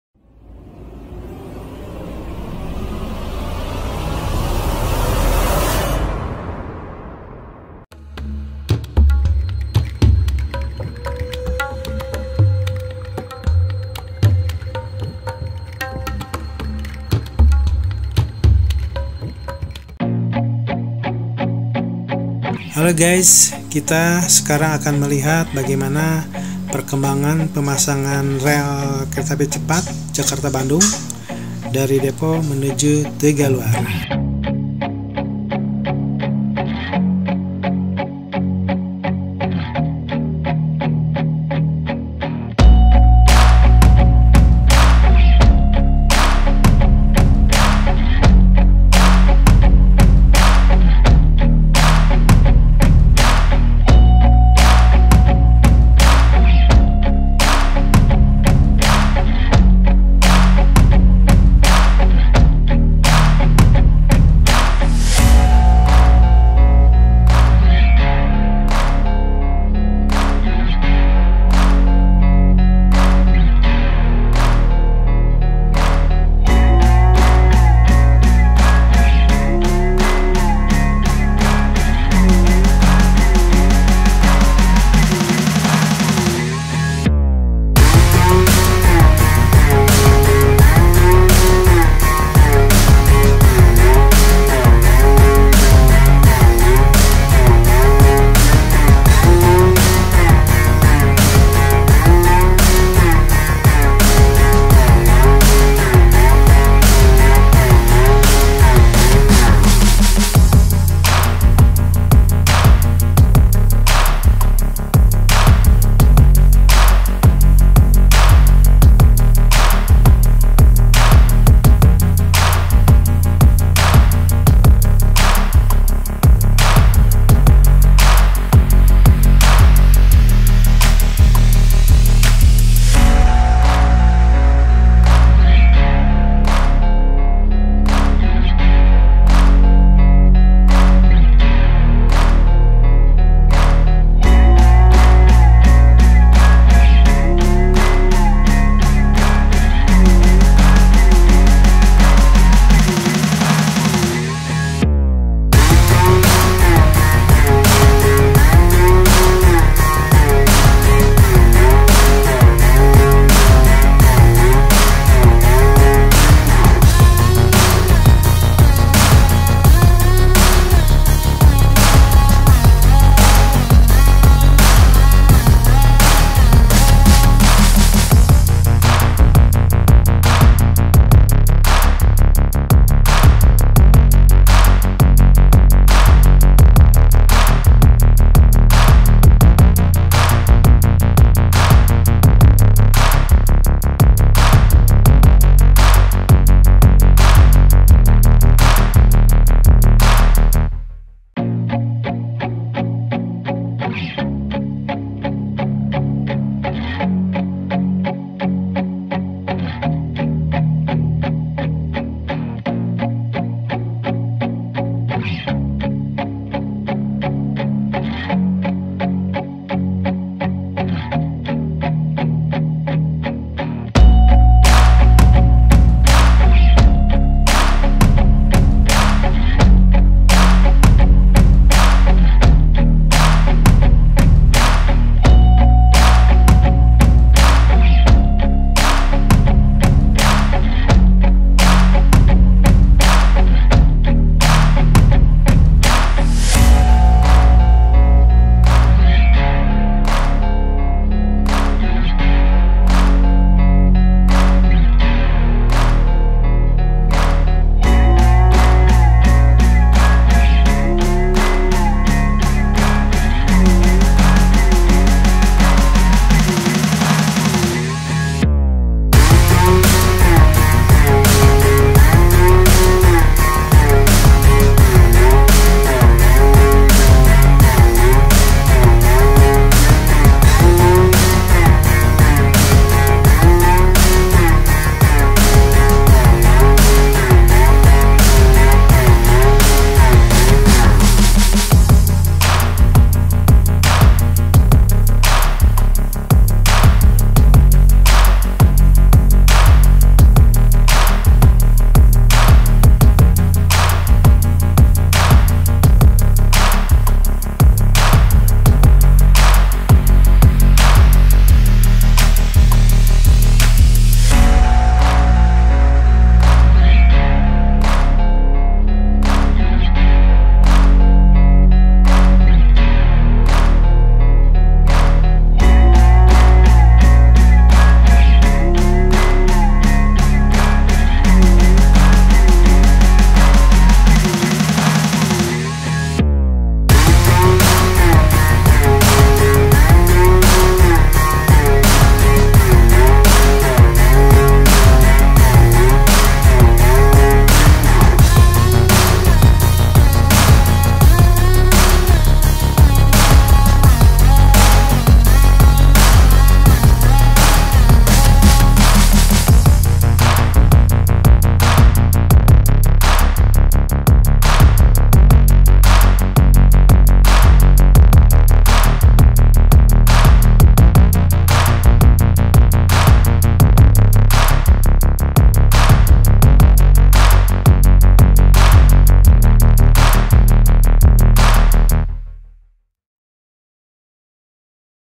Perkembangan terkini Pemasangan Rel Kereta Cepat di Tegalluar
Perkembangan pemasangan rel kereta cepat Jakarta Bandung dari Depo ke Stasiun Tegalluar